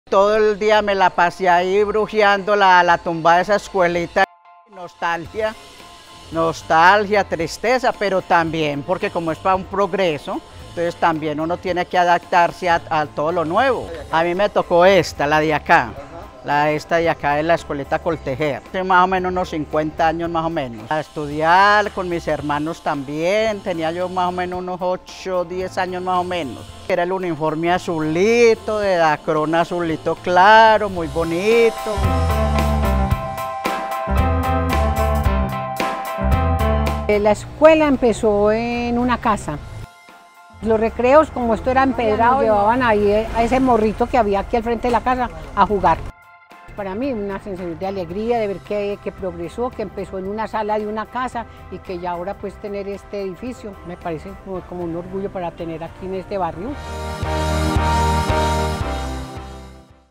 Exalumnas sueñan con la nueva sección escuela Alejando Echavarría
Como a Luz Mery y Amparo, exalumnas de la antigua escuela Alejandro Echavarría, nos emociona ver el 80% de avance de obra de la sección escuela que tendrá 4.199 m² para la educación en nuestra